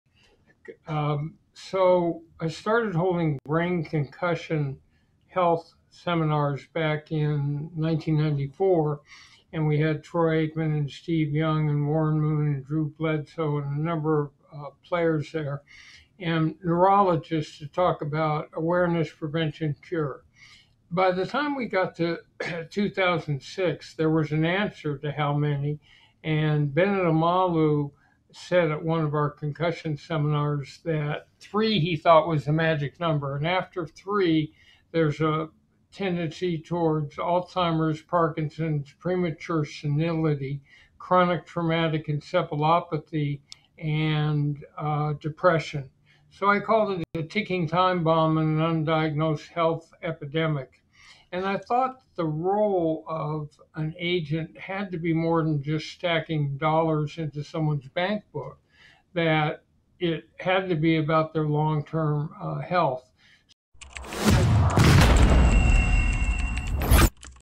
Brain Health: How Concussions Impact Long-Term Health? You Need to Know!
In 1994, concussion health seminars began with football legends and neurologists. By 2006, Dr. Omalu identified three concussions as a tipping point, potentially leading to long-term brain health issues.